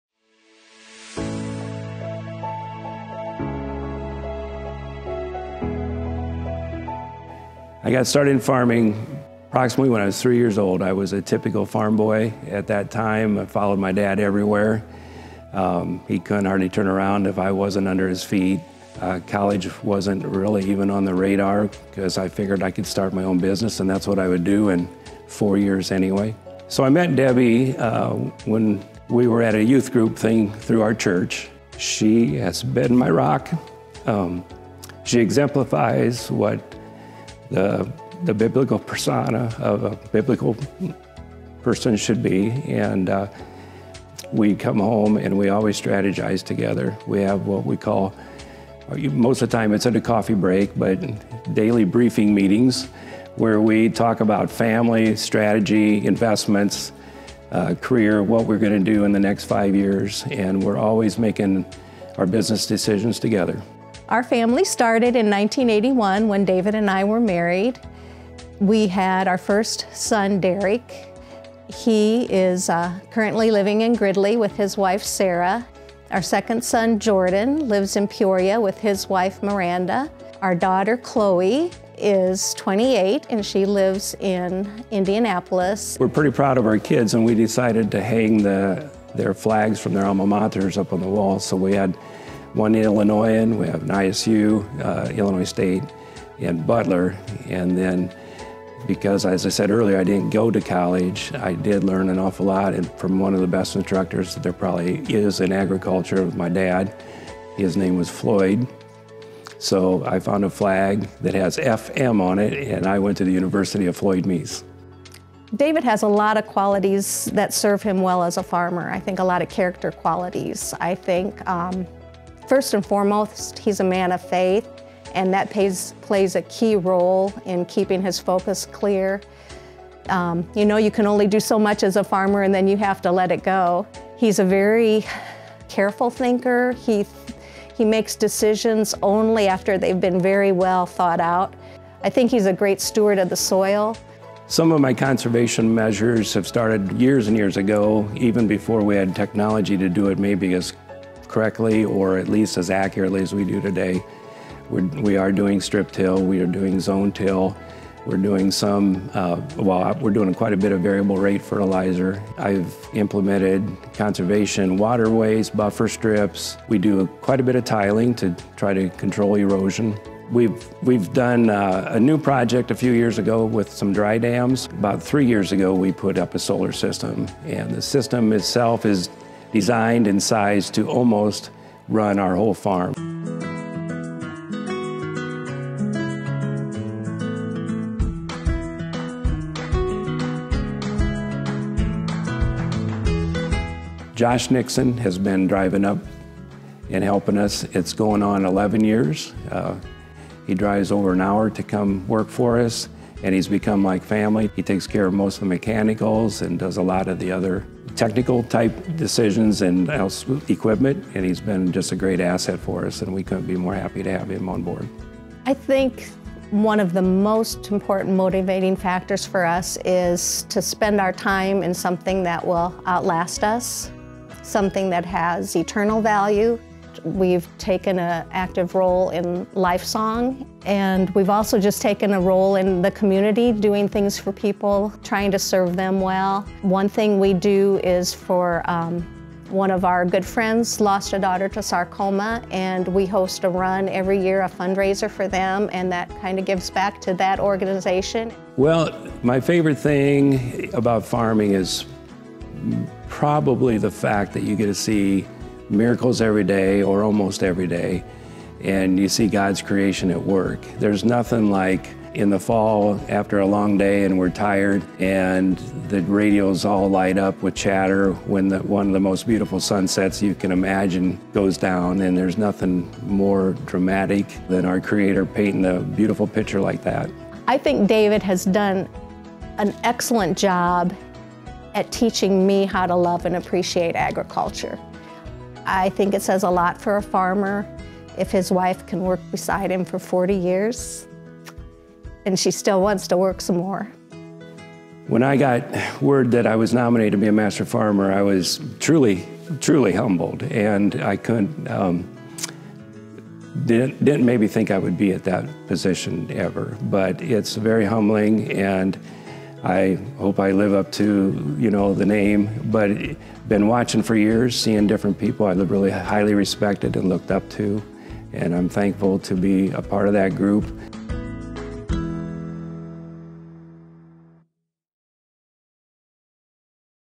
2023 Prairie Farmer Master Farmer: David Meiss
This 2023 Prairie Farmer Master Farmer from Gridley, Ill., is a servant-leader around the farm and for his family, his community and his church.